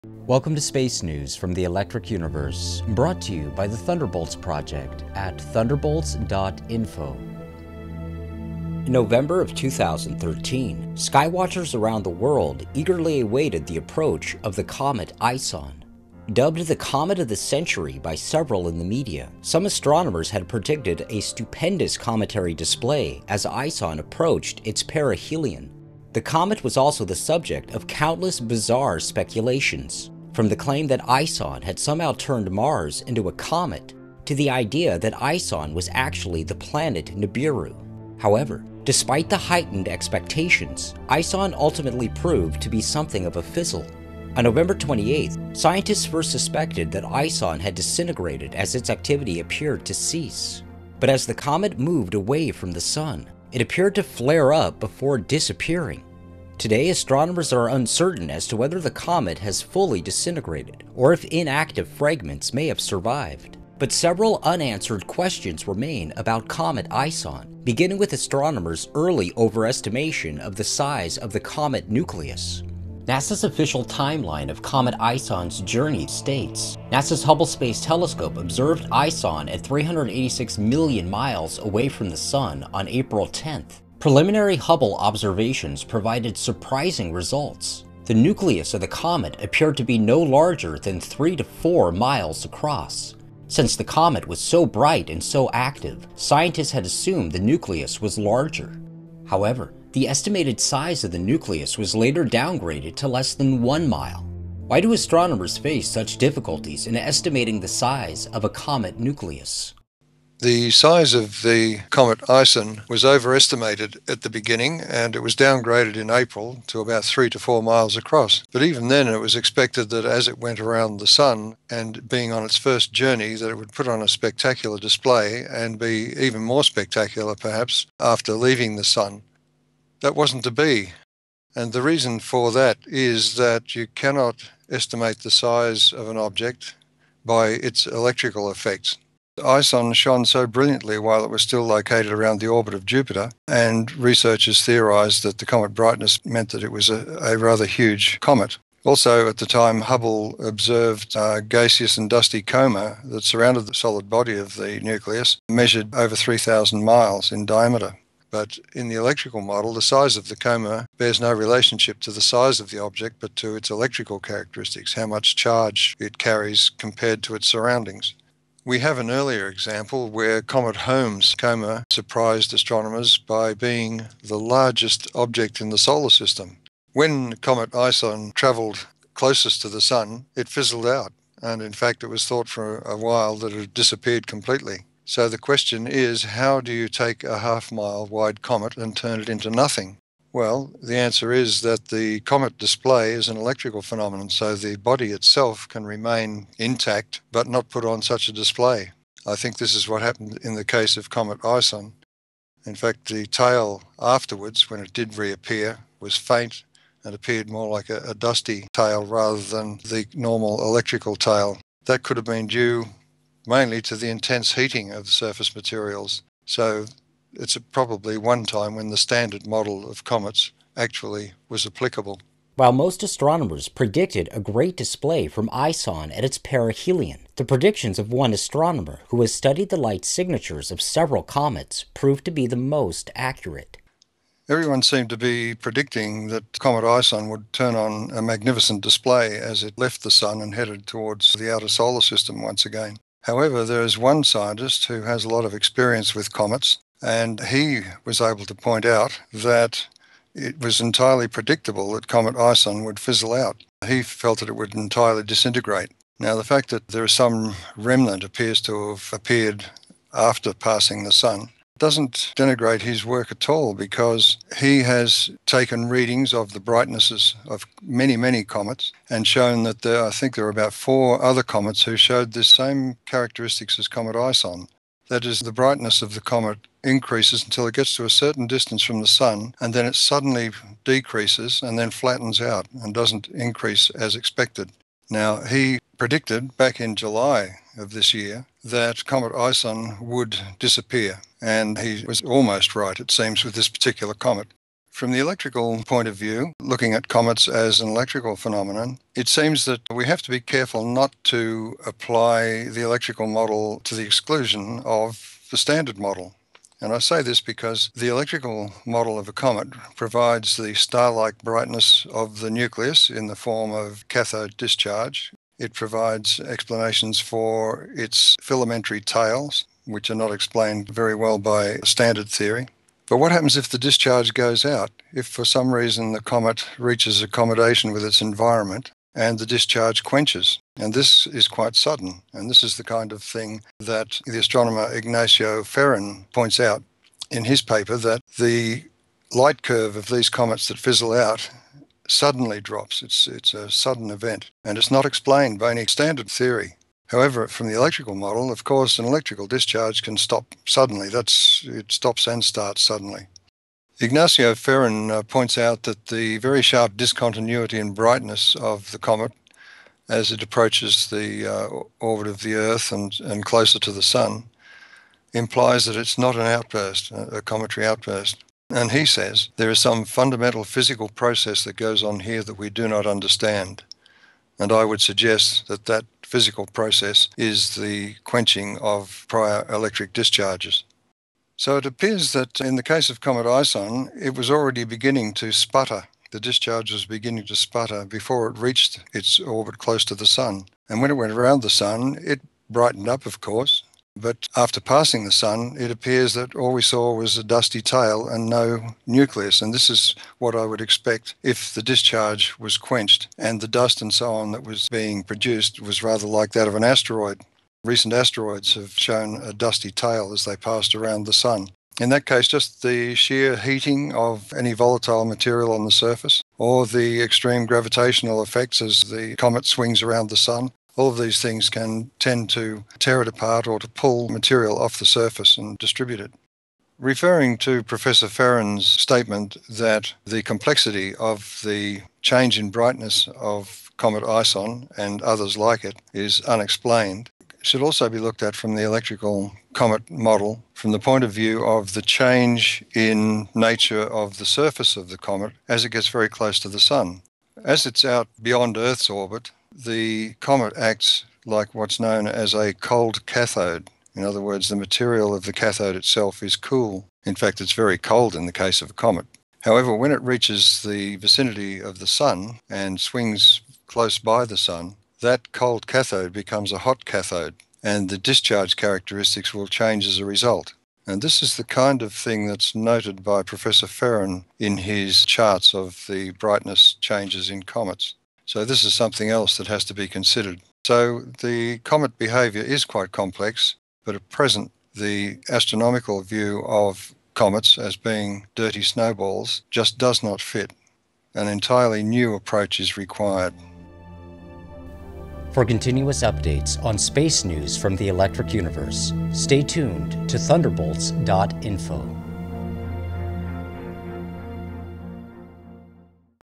Comet ISON Mysteries Continue | Space News
Want to network with other like-minded individuals? Join us at EU2015 Conference: Paths of Discovery, June 25-29, Sheraton Phoenix Downtown.

In November 2013, skywatchers around the world eagerly awaited the approach of Comet ISON.  Dubbed the "Comet of the Century" by several in the media, some astronomers had predicted a stupendous cometary display as Comet ISON approached its perihelion. The comet was also the subject of countless bizarre speculations. However, despite the heightened expectations, ISON ultimately proved to be somewhat of a fizzle.